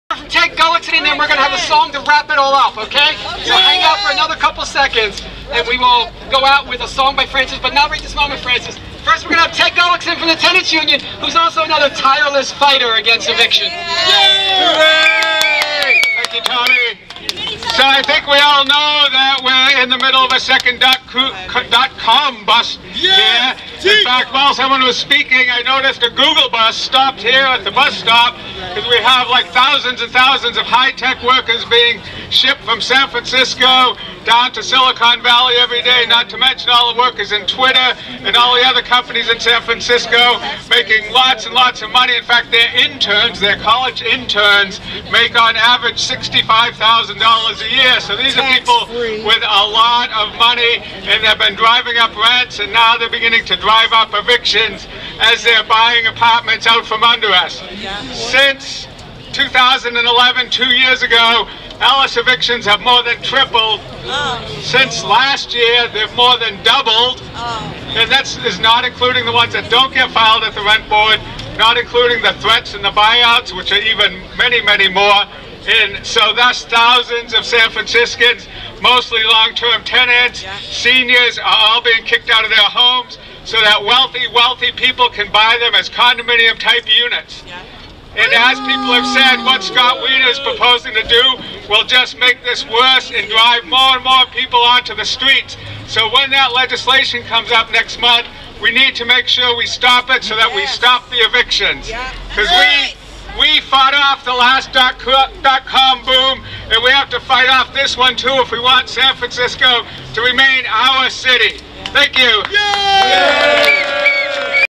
Part XX of "Happy Holidays, Now Get the Hell Out" on December 19, 2012: Stop San Francisco Evictions
Part XX of "Happy Holidays, Now Get the Hell Out" on December 19, 2012: Stop San Francisco Ellis Act Evictions! (VID00020.MOV)